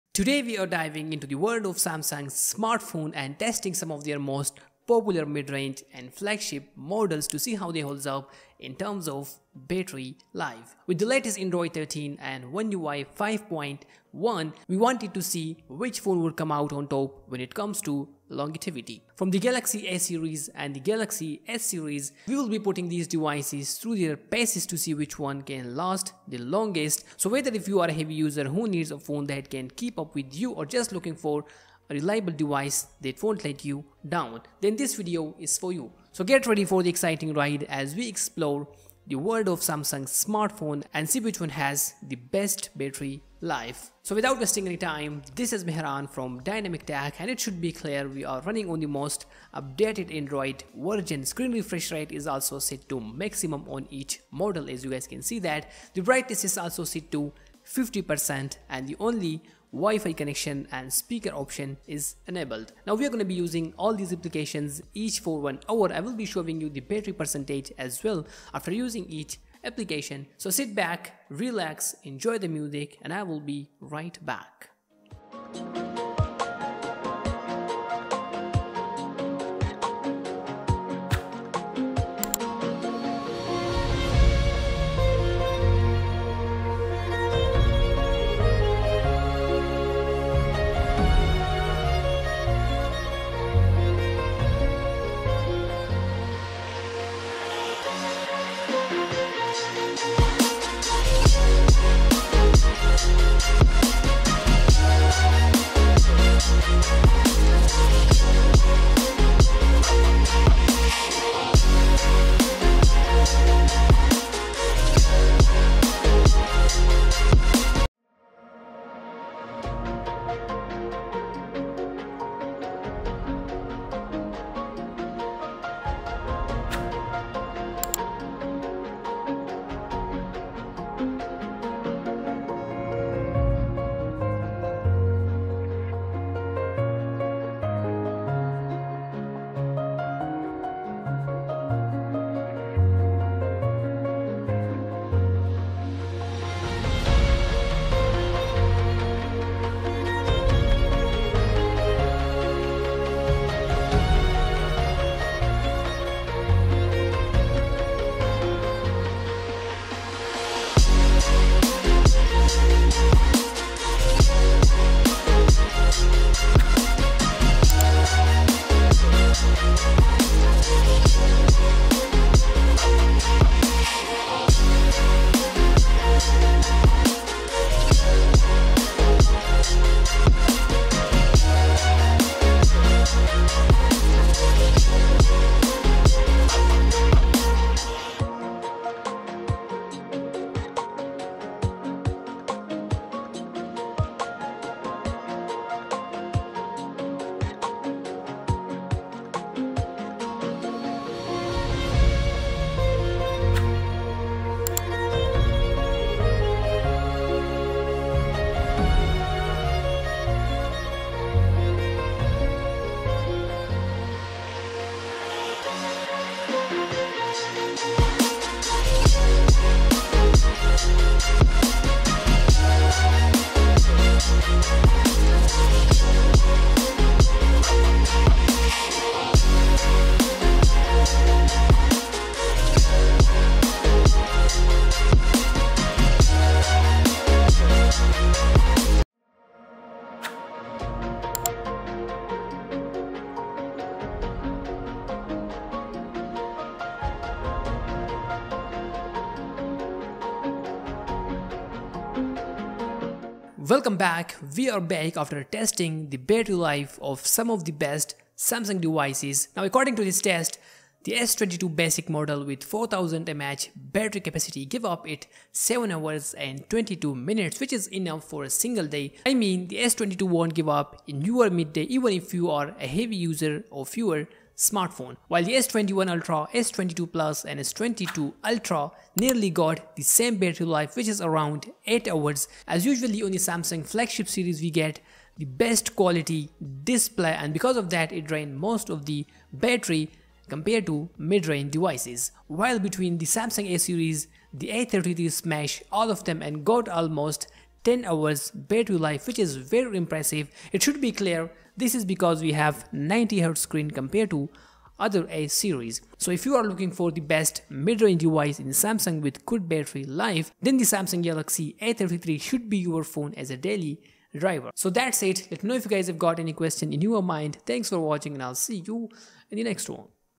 Samsung Battery Life Drain Test in 2023 - A53 vs A33 vs A52s vs A73/S21/S21 Ultra/S22 Plus/S22 Ultra
after Android 13 and One ui 5.1 its time to find out which samsung Smartphone has got the best battery life between samsung galaxy a53 vs samsung galaxy a33 vs samsung galaxy a52s vs samsung galaxy a73 vs samsung galaxy s21 vs samsung galaxy s21 ultra vs samsung galaxy s22 plus vs samsung galaxy s22 ultra in 2023, we will be pushing them to their limit in this battery life test to find out the which one has the best battery life in 2023.